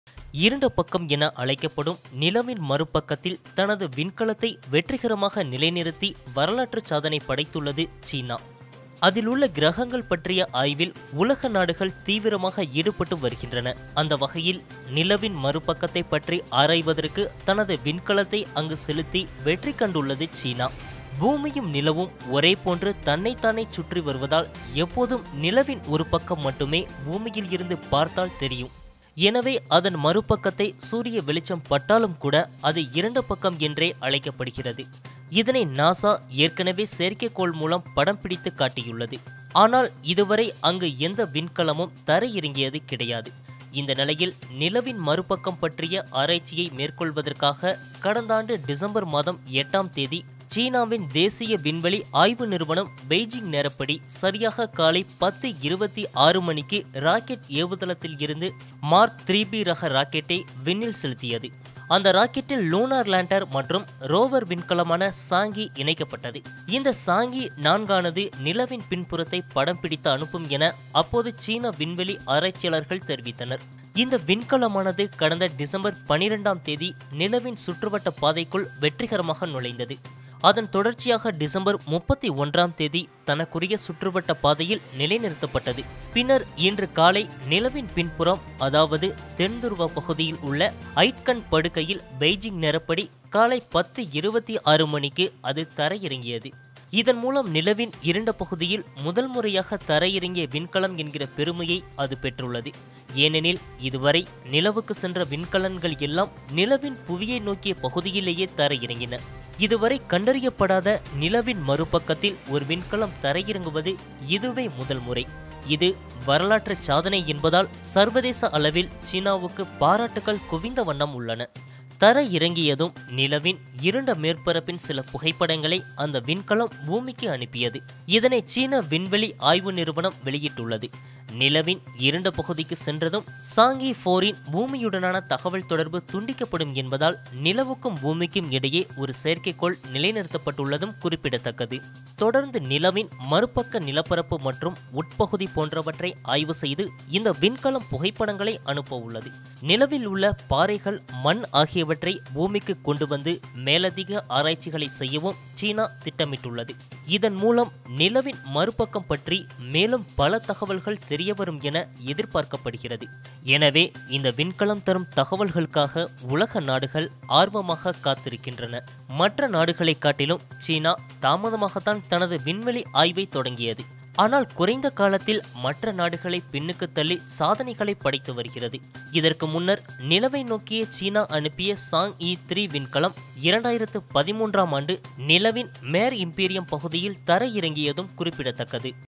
China Lands Spacecraft On Far Side Of Moon : நிலவின் இருண்ட பக்கத்தில் தரையிறங்கி சாதனை படைத்த சீனா
இருண்டபக்கம் என அழைக்கப்படும் நிலவின் மறுபக்கத்தில் தனது விண்கலத்தை வெற்றிகரமாக நிலைநிறுத்தி வரலாற்றுச் சாதனை படைத்துள்ளது சீனா. விண்வெளி, அதில் உள்ள கிரகங்கள் பற்றிய ஆய்வில் உலக நாடுகள் தீவிரமாக ஈடுபட்டு வருகின்றன.

In an historic first, China has successfully landed a rover on the far side of the moon, Chinese state media announced Thursday, a huge milestone for the nation as it attempts to position itself as a leading space power.